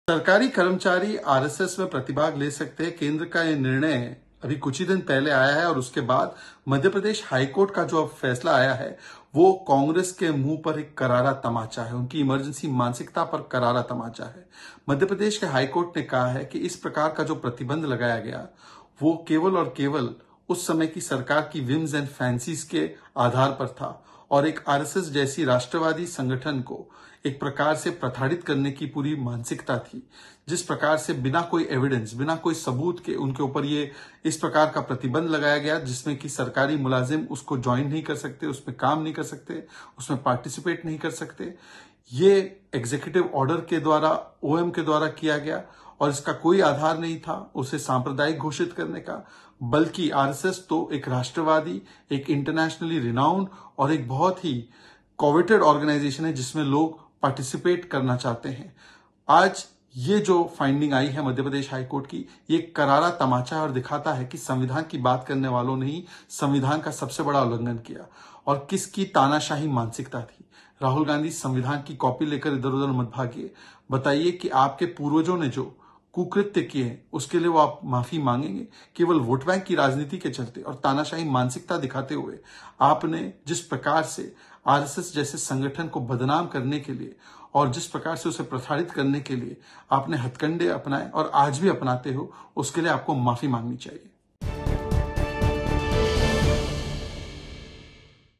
Rahul Gandhi को अपने पूर्वजों की गलती के लिए माफी मांगनी चाहिए: Shehzad Poonawalla
आरएसएस पर मध्य प्रदेश हाई कोर्ट के फैसले पर बीजेपी के राष्ट्रीय प्रवक्ता शहजाद पूनावाला ने कहा, सरकारी कर्मचारी आरएसएस के कार्यक्रम में भाग ले सकते हैं। केंद्र सरकार का यह फैसला कुछ दिन पहले ही आया है और उसके बाद मध्य प्रदेश हाई कोर्ट का फैसला कांग्रेस के लिए कड़ी फटकार है । उन्होंने कहा, मध्य प्रदेश हाई कोर्ट ने कहा है इस प्रकार का जो प्रतिबंध लगाया गया वो केवल उस समय की सरकार के आधार पर था और आरएसएस जैसी राष्ट्रवादी संगठन को एक प्रकार से प्रताड़ित करने की मानसिकता थी । शहजाद पूनावाला ने कहा, राहुल गांधी संविधान की कॉपी लेकर इधर उधर मत भागीये ये बताये की आपके पूर्वजों ने जो कूकृत्य किये हैं उसके लिए आप माफी मांगेंगे केवल वोट बैंक की राजनीति के चलते तानाशाही मानसिकता दिखाते हुए आपने आरएसएस जैसे संगठन को बदनाम करने के लिए हथकंडे अपनाएं उसके लिए आपको माफी मांगनी चाहिए।